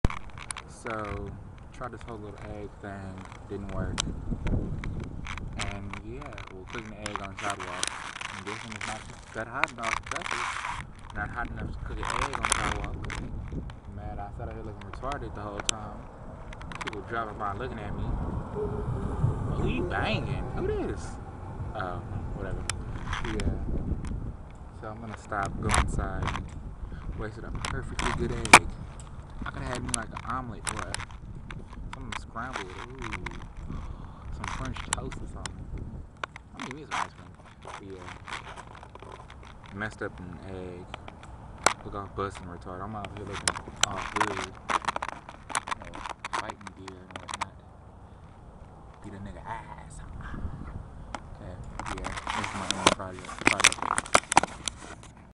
Cooking Outside
So It Was Like Hella Not And Desided To Try To Cook And Egg Out Side.